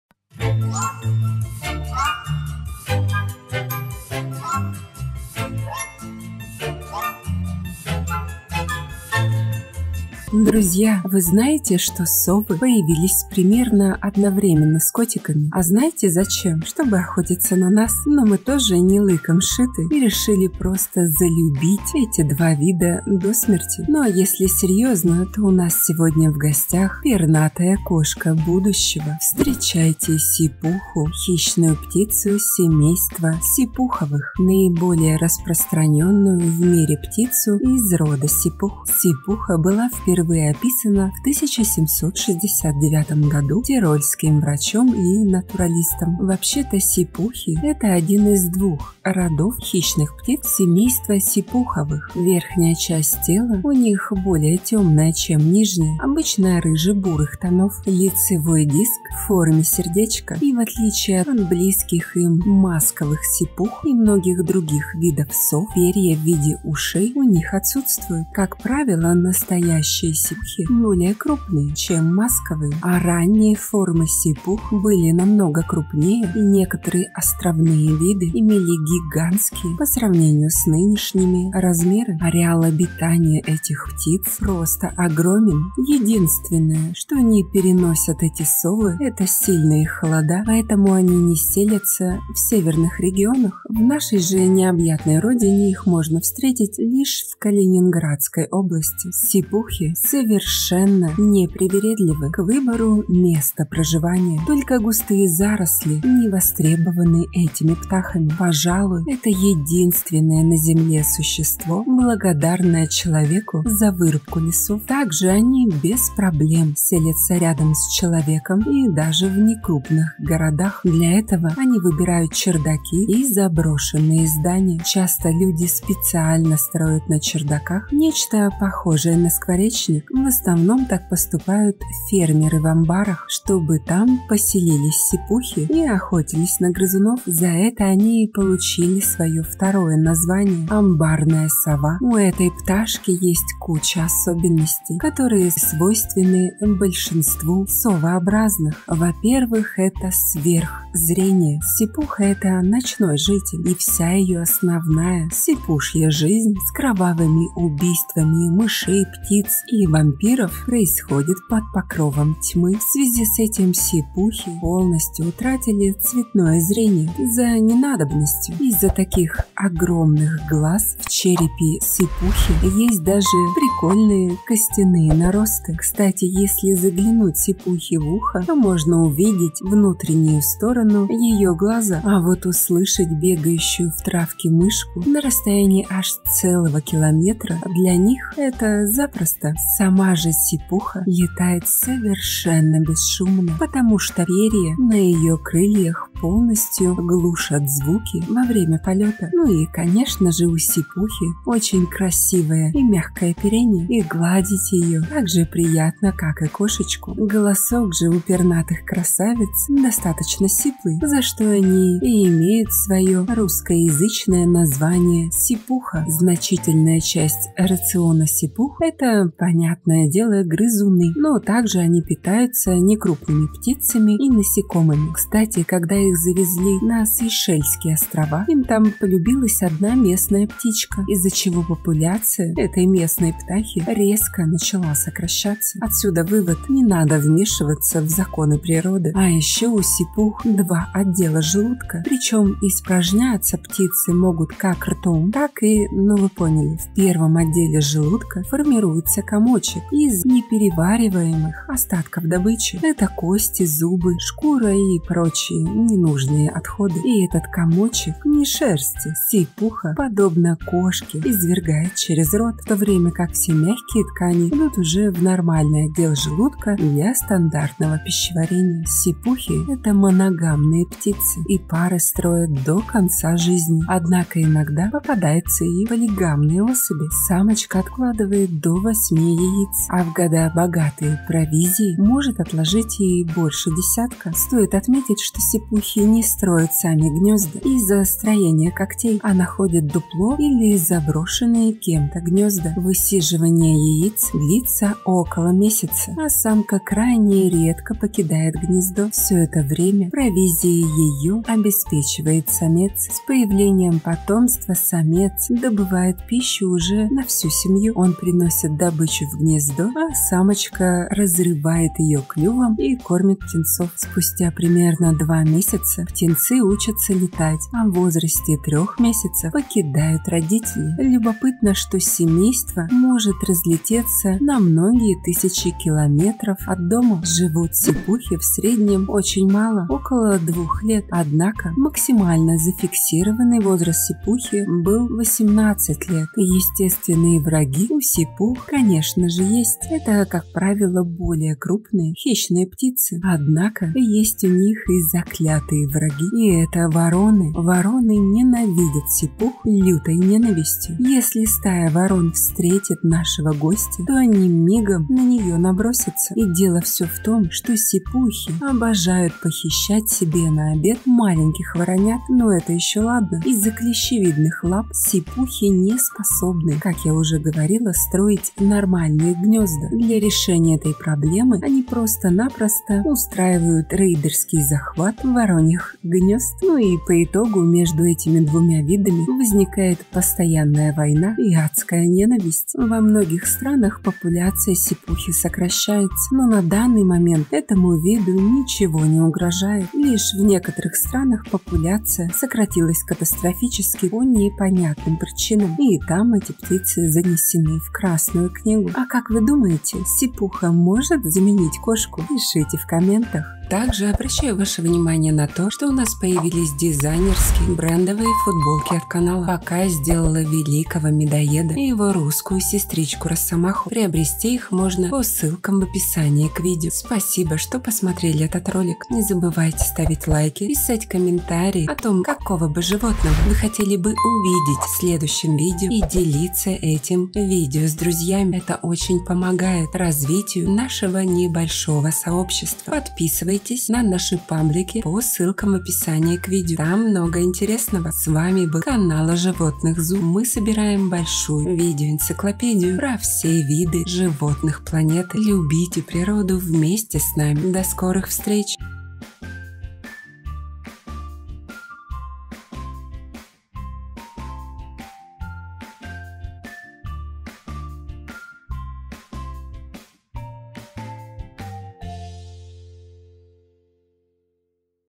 СИПУХА: Сова с мордочкой в форме сердца | Интересные факты про сов
Добро пожаловать на канал Zoo! Мы собираем большую видео Энциклопедию про ВСЕ виды Животных Планеты.

Встречайте сипуху (Tyto alba) — хищную птицу семейства сипуховых, наиболее распространённую  в мире птицу из рода сипух. Сипуха была впервые описана в 1769 году тирольским врачом и натуралистом.